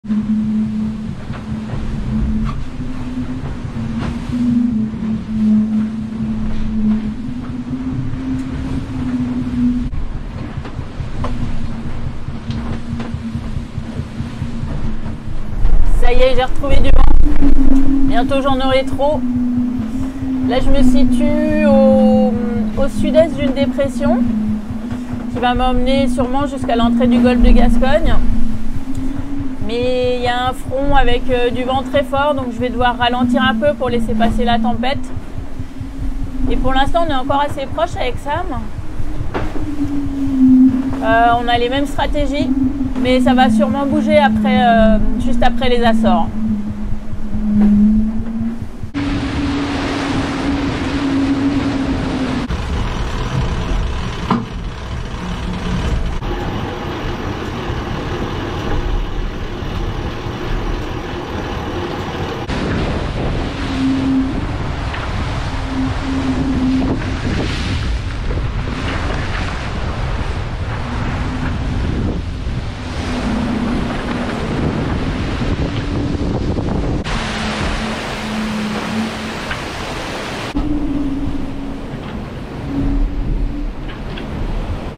Message du bord - Jeudi 18 février 2021
💬  "Je me situe au sud-est d’une dépression qui va m’emmener au moins jusqu’à l’entrée du Golfe de Gascogne. Mais il y a un front avec du vent très fort donc je vais devoir ralentir un peu pour laisser passer la tempête…"

Changement de rythme à bord de l’IMOCA MACSF !
Après plusieurs jours de pétole, le voilier rouge et blanc s’est libéré de la torpeur de l’anticyclone et glisse désormais sur l’Océan Atlantique à une vitesse moyenne de 15 nœuds !
👉 Avant de nous quitter, un petit challenge : saurez-vous retrouver Sophie la girafe dans le vidéo ? 😉🦒